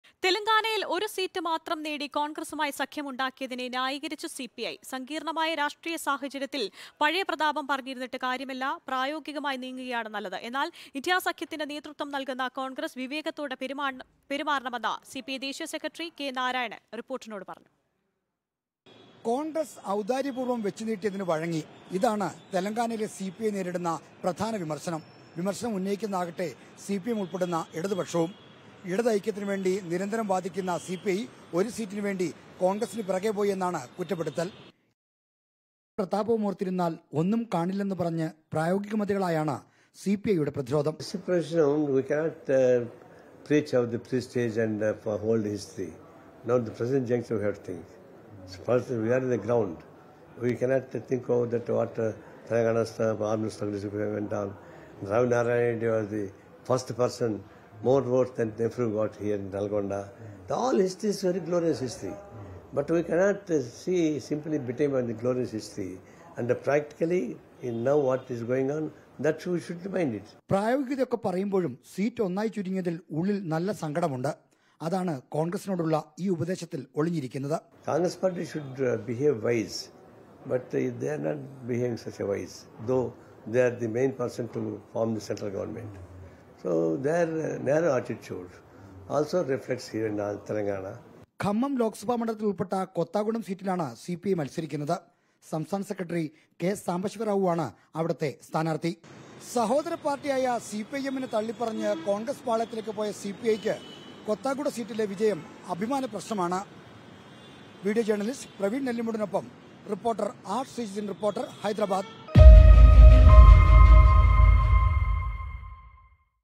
തെലങ്കാനയില്‍ ഒരു സീറ്റ് മാത്രം നേടി കോണ്‍ഗ്രസുമായി സഖ്യം; ന്യായീകരിച്ച് സിപിഐ
തെലങ്കാനയില്‍ ഒരു സീറ്റ് മാത്രം നേടി കോണ്‍ഗ്രസുമായി സഖ്യം; ന്യായീകരിച്ച് സിപിഐ | Telangana Politics

With Regards
Team RBC